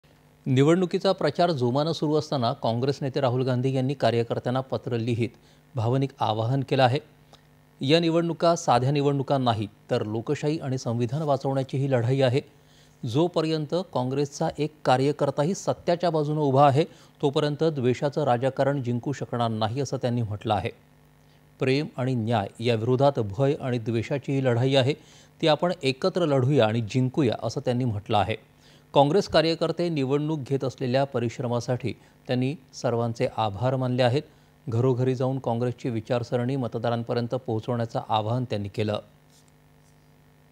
काँग्रेस नेते राहुल गांधी यांनी कार्यकर्त्यांना पत्र लिहत भावनिक आवाहन.
दि.०६.०५.२०२४ :

     निवडणुकीचा प्रचार जोमानं सुरु असताना,काँग्रेस नेते राहुल गांधी यांनी कार्यकर्त्यांना पत्र लिहत भावनिक आवाहन केलं आहे. या निवडणुका साध्या निवडणुका नाहीत, तर लोकशाही आणि संविधान वाचवण्याची ही लढाई आहे. जोपर्यंत काँग्रेसचा एक कार्यकर्ताही सत्याच्या बाजुनं उभा आहे, तोपर्यंत द्वेषाचं राजकारण जिंकू शकणार नाही, असं त्यांनी म्हटलं आहे. प्रेम आणि न्याय या विरोधात भय आणि द्वेषाची ही लढाई आहे. ती आपण एकत्र लढुया आणि जिंकुया, असं त्यांनी म्हटलं आहे. काँग्रेस कार्यकर्ते निवडणुक घेत असलेल्या परिश्रमासाठी त्यांनी सर्वांचे आभार मानले आहेत. घरोघरी जाऊन काँग्रेसची विचारसरणी मतदारांपर्यंत पोहचवण्याचं आवाहन त्यांनी केलं आहे.

8.30am | 11.00am | 1.00pm | 4.30pm | 7.00pm | 9.30pm